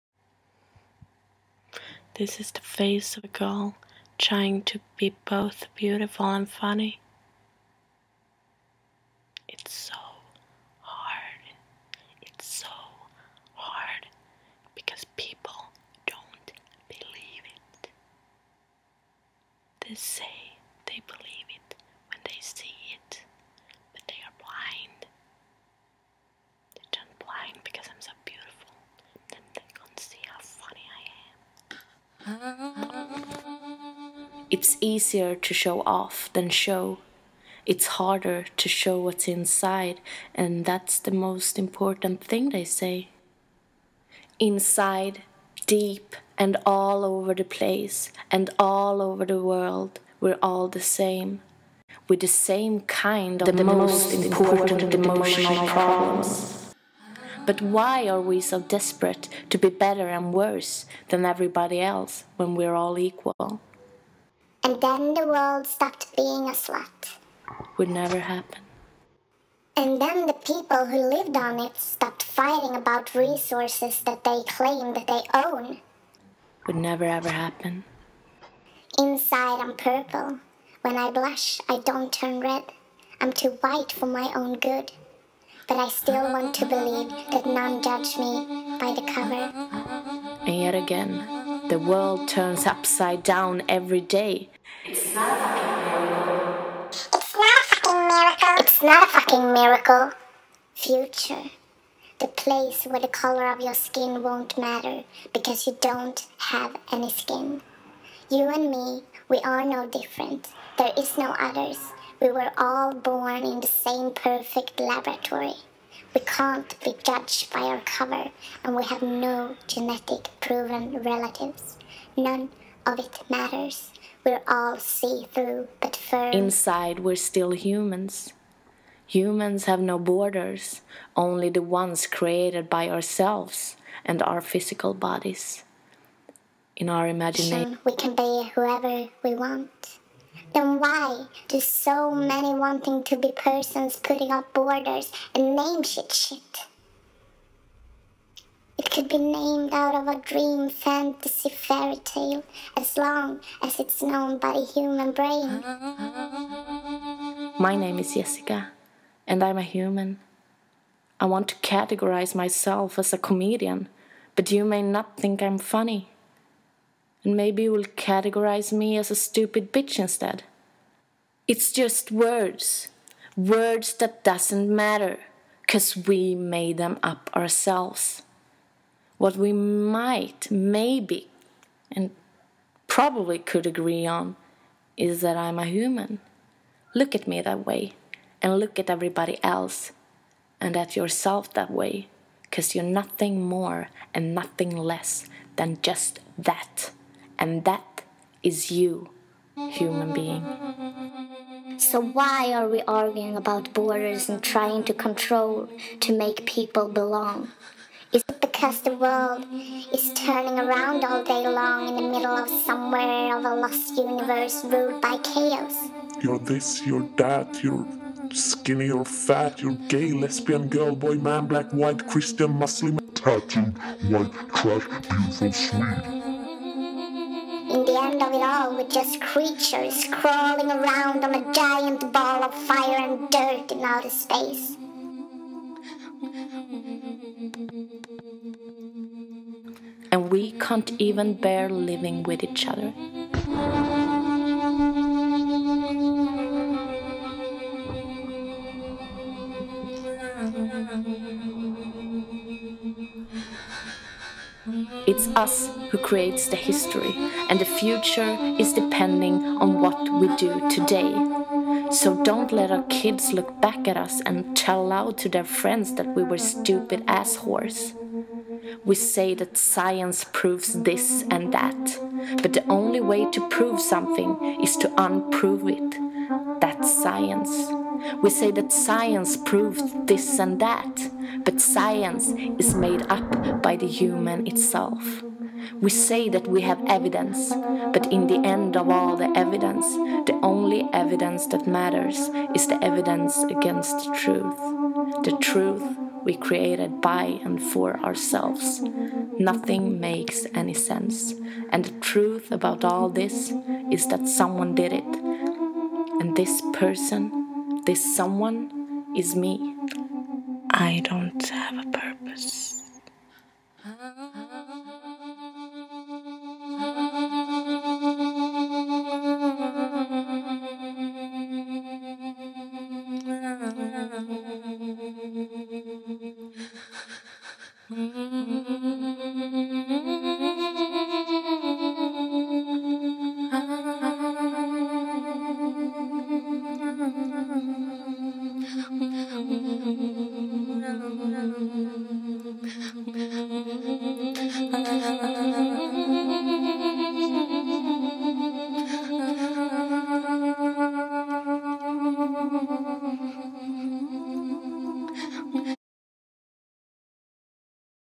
POOR PEOPLE VS HUMAN BEINGS
Part of Artshow launched Los Angeles 6th of december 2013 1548 S Central Ave.
Totaly anti- racism, non- gender .
PRO LOVE ! ! ! NO BORDERS! ! ! FREE THEM SPIRITS! ! !

How hard can it be?